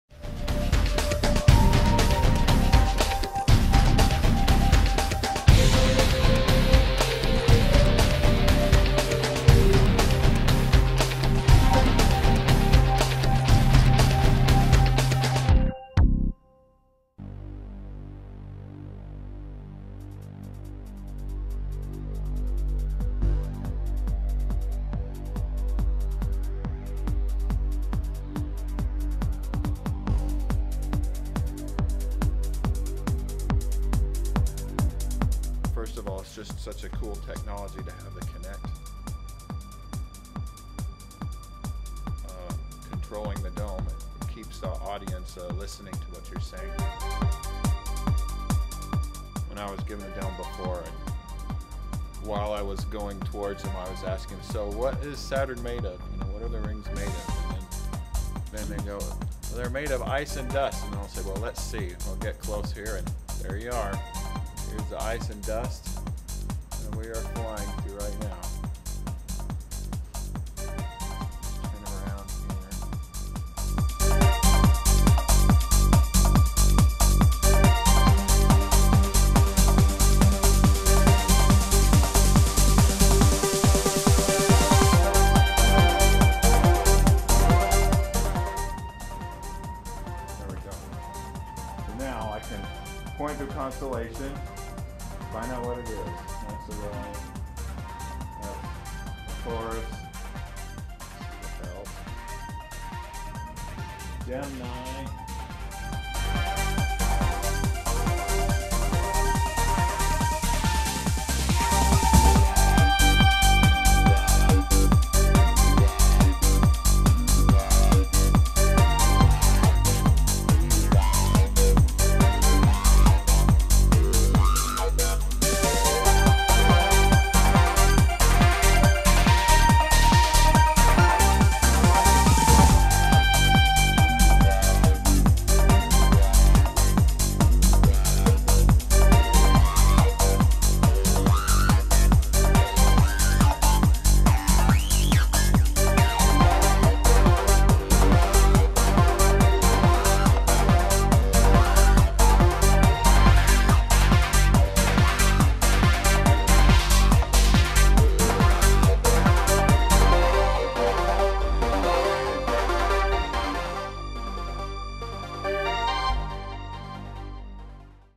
Digistar 4 with Kinect Promo
E&S introduces a revolutionary new controller-free digital planetarium using the XBOX 360 Kinect and Digistar 4.